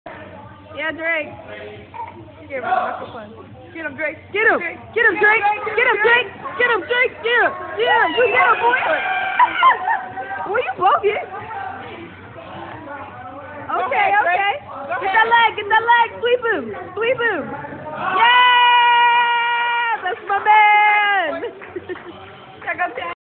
Drake going for the takedown!
My 5yr old a few months into Brazilian Jiujitsu has no fear!!